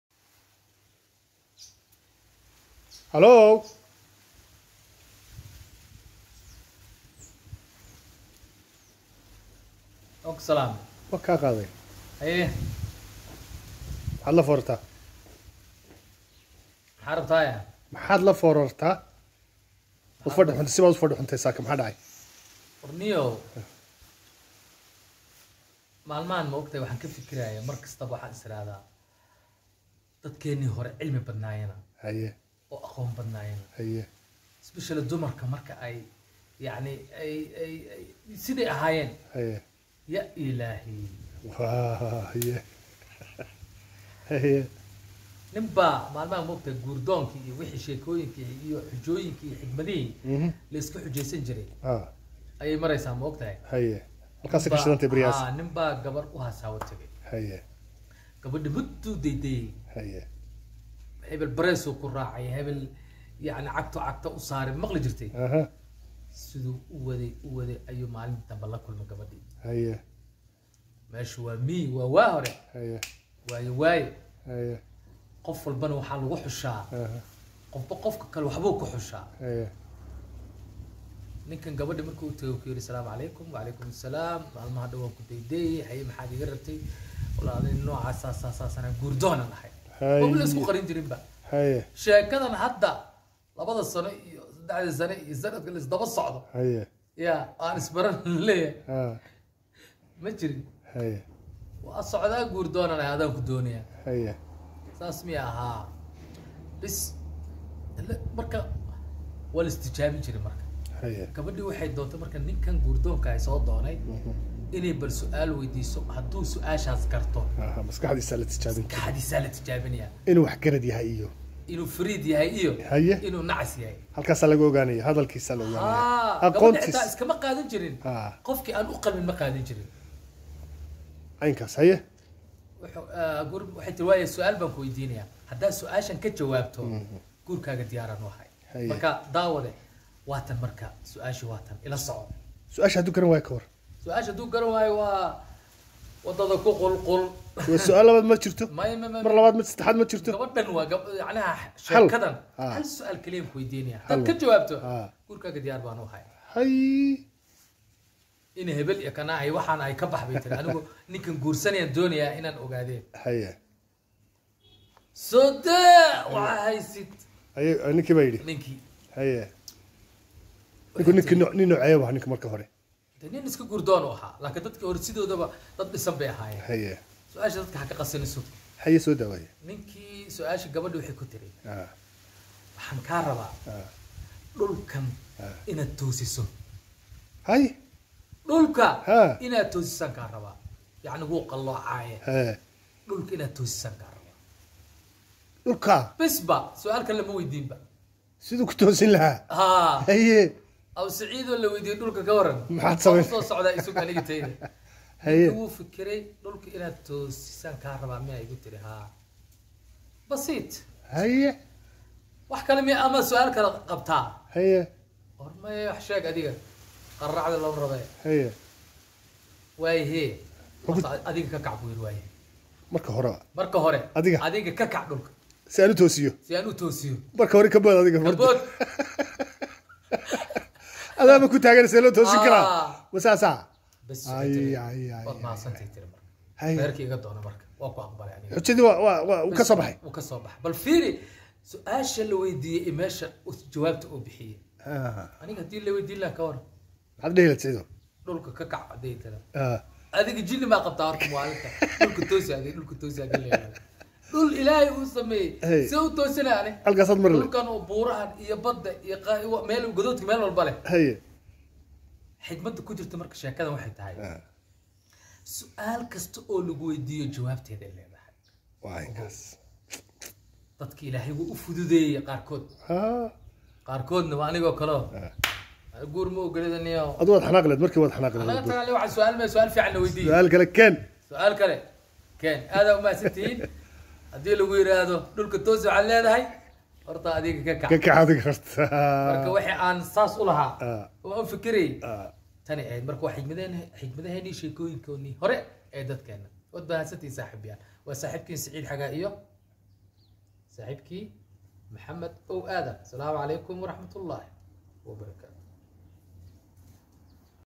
Guurdoonkii Garaadkiisu Badbaadiyey| Halxidhaale| Siciid Xagaa Afrax
Siciid Xagaa Afrax, waa suxufi iyo sheekokasheeke soo bandhigaya murtida af Soomaaliga. waxa uu hadda soo bandhigayaa halxidhaale garaadhubin ah, oo gabadh caqli badani weydiisay nin guur u soo bandhigay.